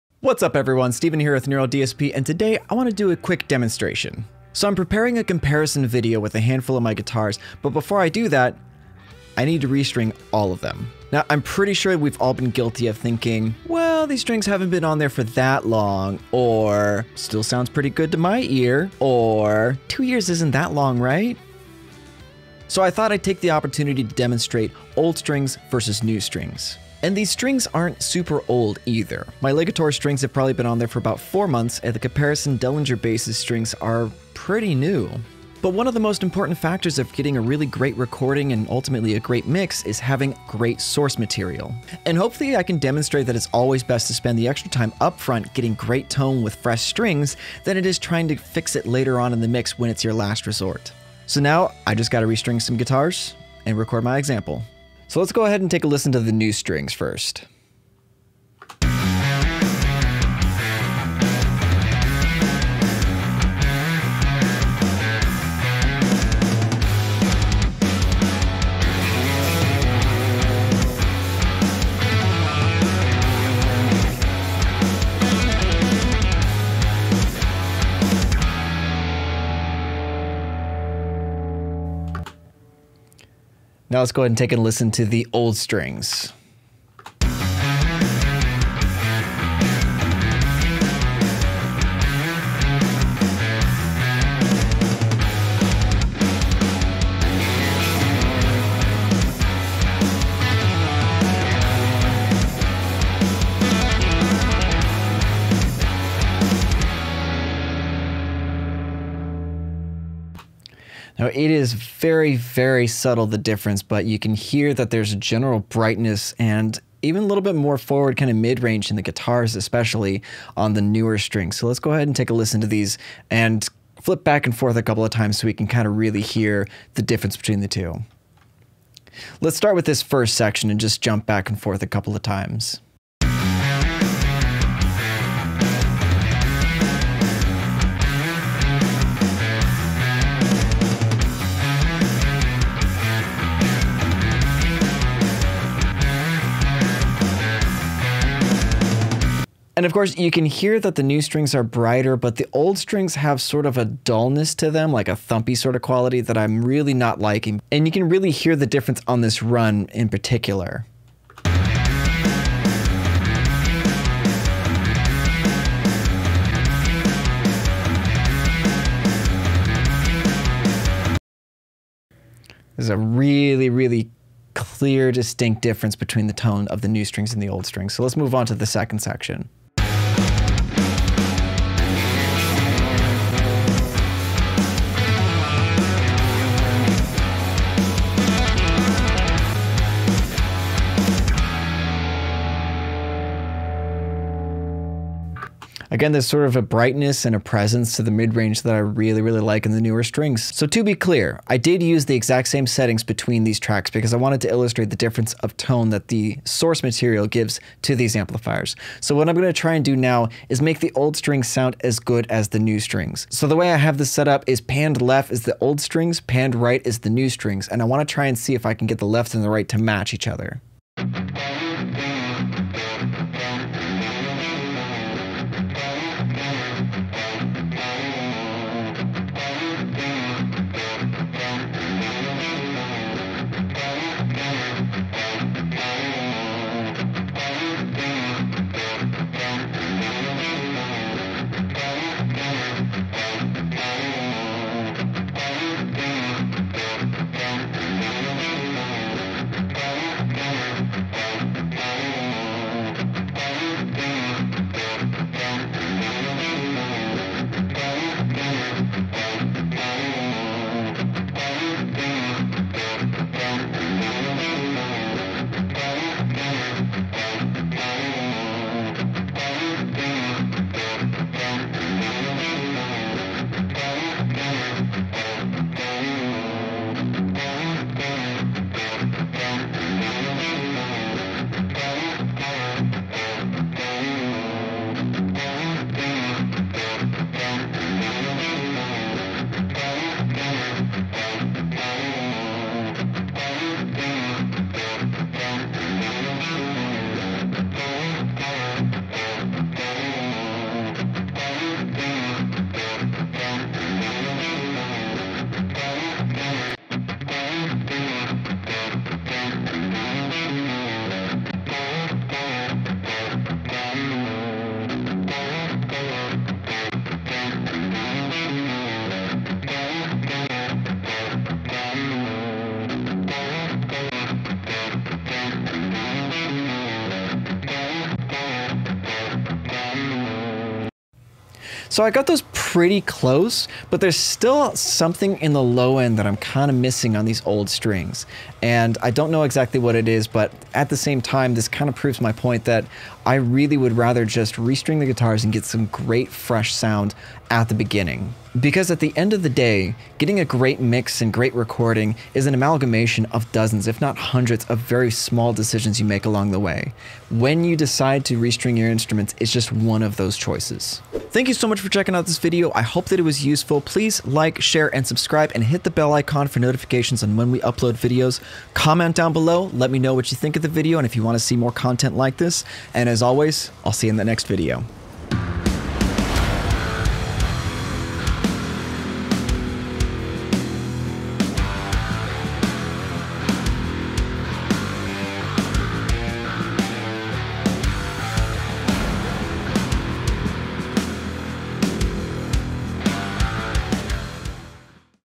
How Much of a Difference Do New Strings Make to Your Mix?
In this video, Steven compares how much a difference new strings can make to the overall tone of the mix when recording your guitars.

Try the Neural DSP Archetype: Plini today!